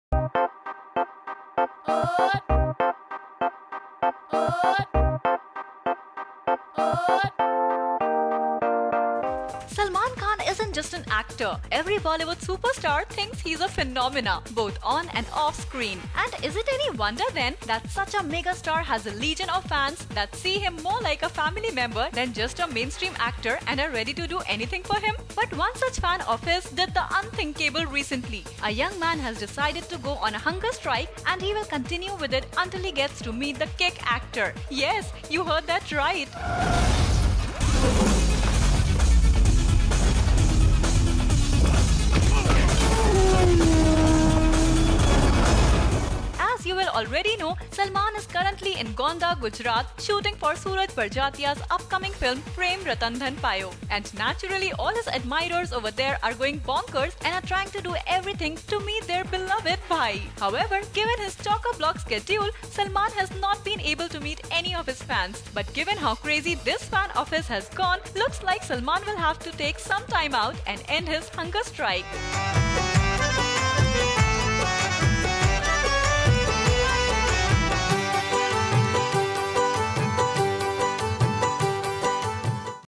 Salmans fan goes on a hunger strike
Salman Khan isn’t just an actor. Every Bollywood superstar thinks he’s a phenomenon, both on and off screen. And is it any wonder then that such a megastar has a legion of fans who see him more like a family member than just a mainstream actor and are ready to do anything for him.